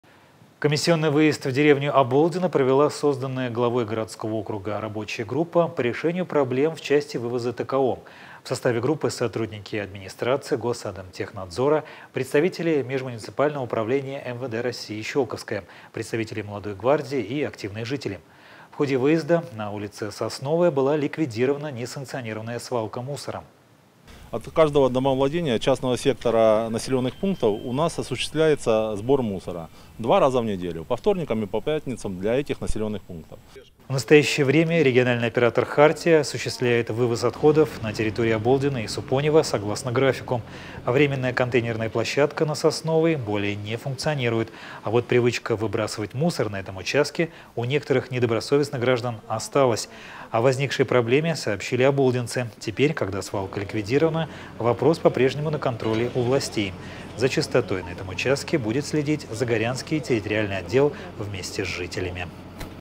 КОНТЕЙНЕРНАЯ ПЛОЩАДКА НА СОСНОВОЙ В ОБОЛДИНО ЛИКВИДИРОВАНА | ТЕПЕРЬ ТОЛЬКО ПАКЕТИРОВАННЫЙ СБОР
В настоящее время региональный оператор Хартия осуществляет вывоз отходов на территории Оболдино и Супонево согласно графику. Временная контейнерная площадка на Сосновой более не функционирует.